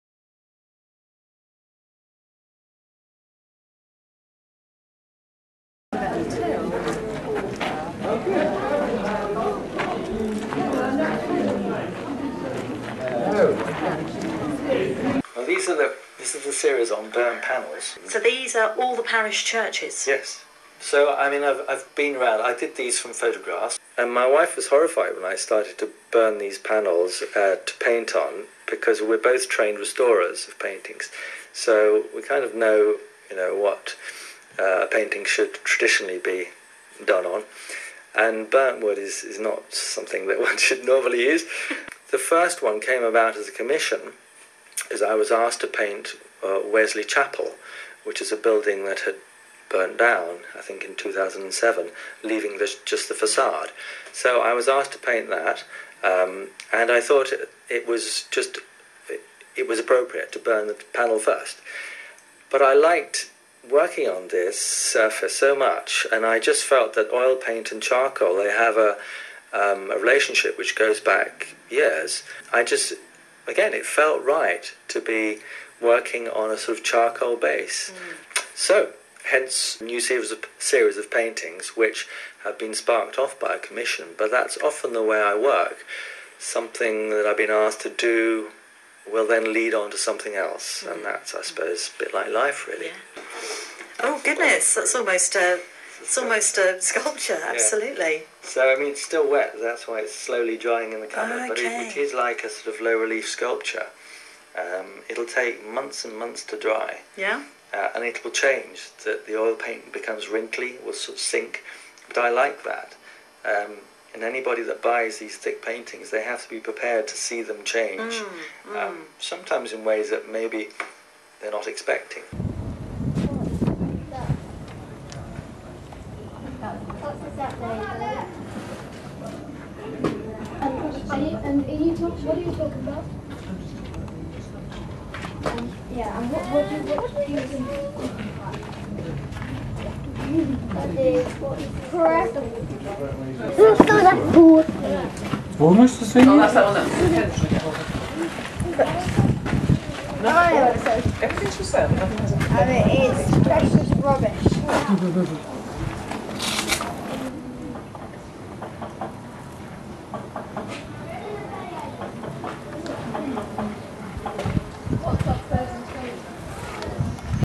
Ian Rolls at Hamptonne
Jersey artist Ian Rolls talks about his work at an exhibition held in 2010 at Hamptonne